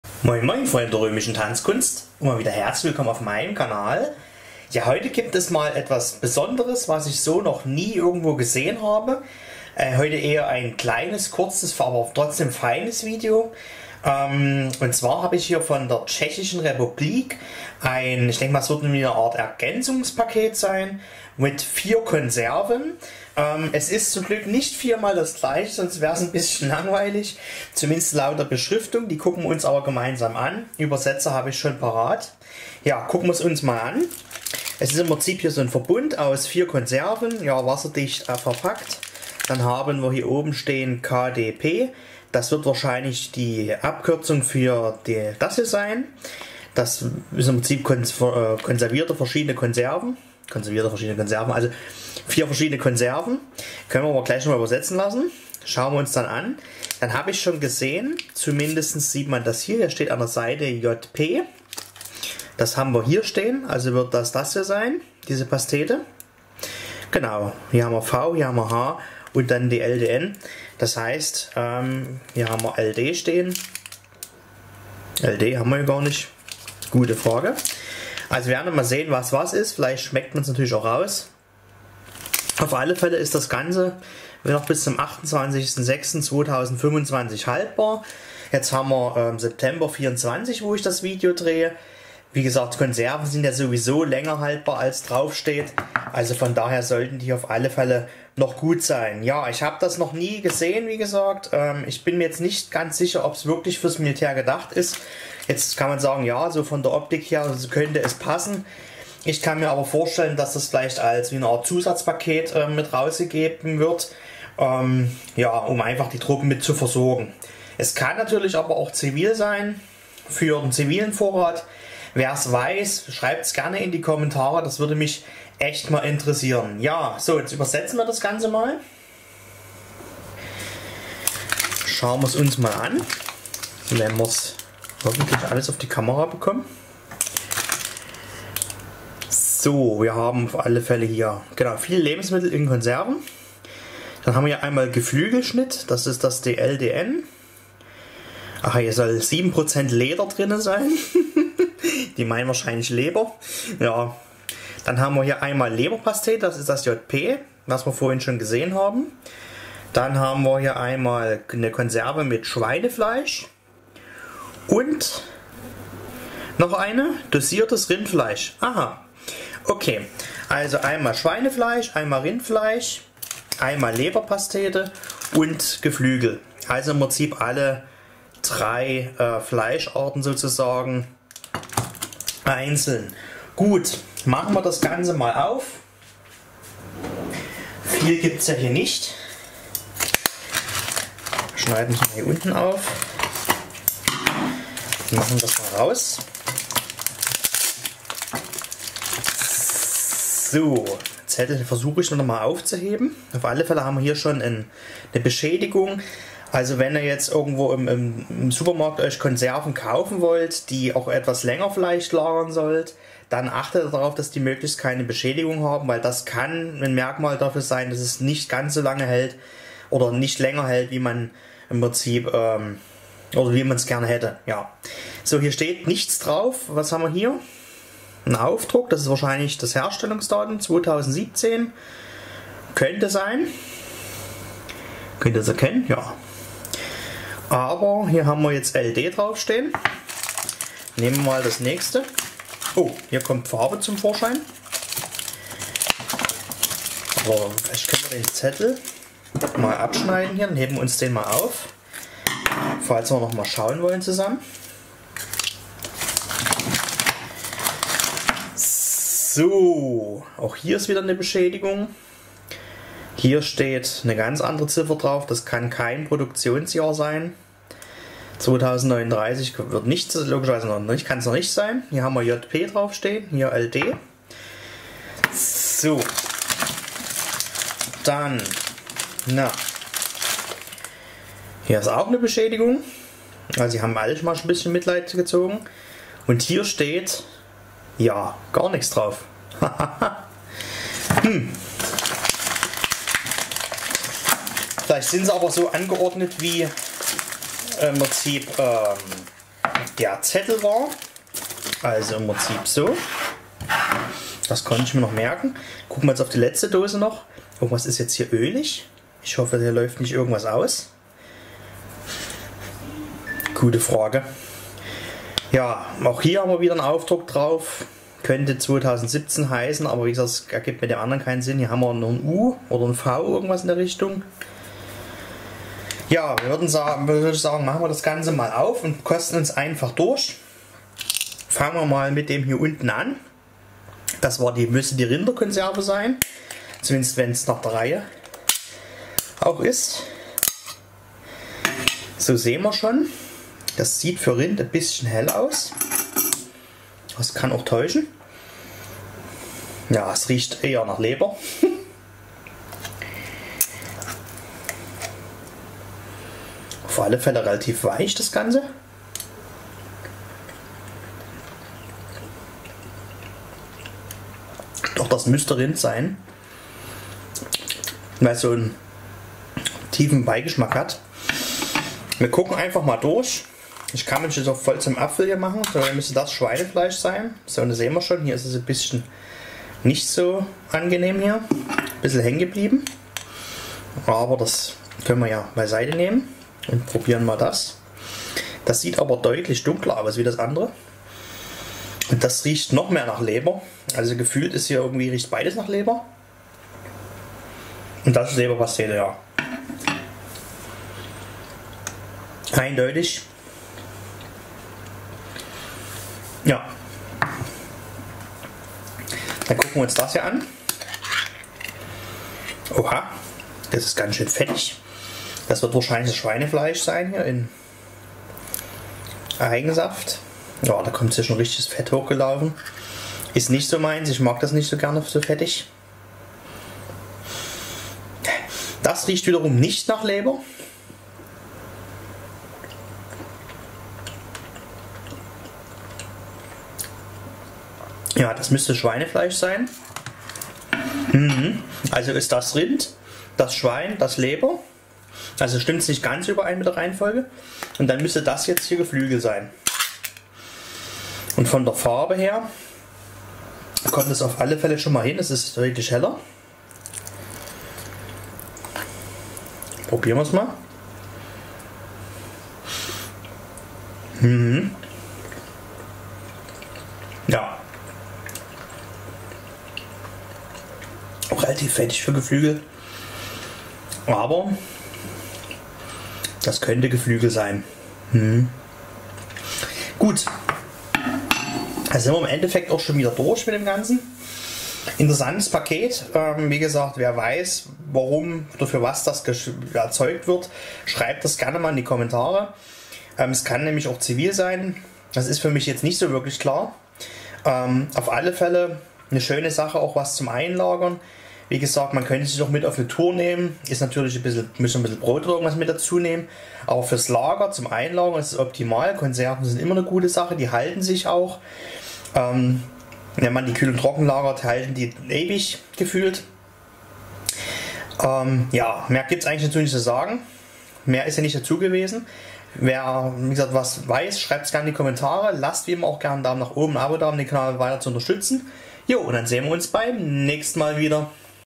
CZ - KDP (MHD 2025)
Hallo in die Runde - heute mal etwas außergewöhnliches was ich so noch nie gesehen habe und nun zum ersten mal erwerben durfte. Ich vermute mal es wird eine Art Zusatzpaket für die Tschechischen Truppen sein - wer es weiss schreibt es gerne in die Kommentare

Lasst gern nen ABO da um den Kanal weiter zu unterstützen und schaltet auch beim nächsten mal  wieder ein :-)